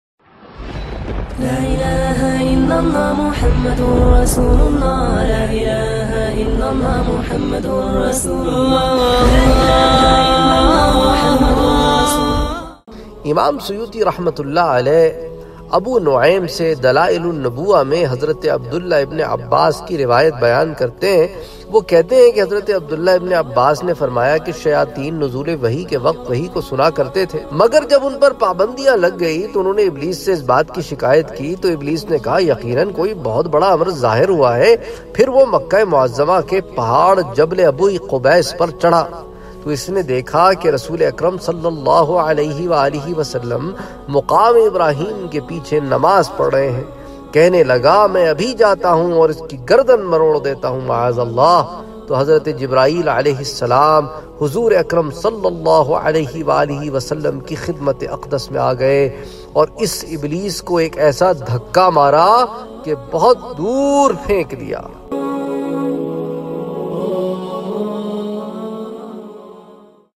Hazrat Jibrael AS nay Iblees ko utha kar Phainka
This lecture is about Hazrat Jibrael AS nay Iblees ko utha kar Phainka